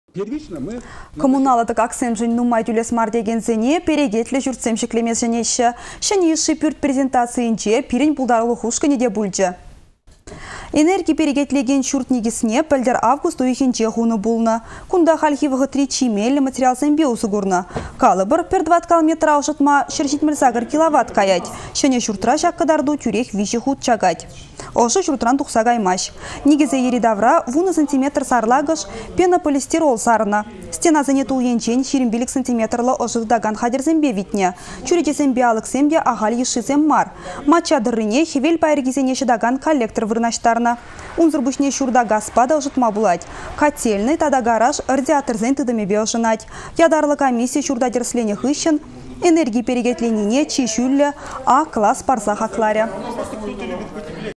Çĕнĕ йышши пÿрт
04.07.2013
Коммуналлă тăкаксемшĕн нумай тÿлес мар текенсене перекетлĕ çуртсем çĕклеме сĕнеççĕ. Çĕнĕ йышши пÿрт презентацийĕнче пирĕн пултарулăх ушкăнĕ те пулчĕ.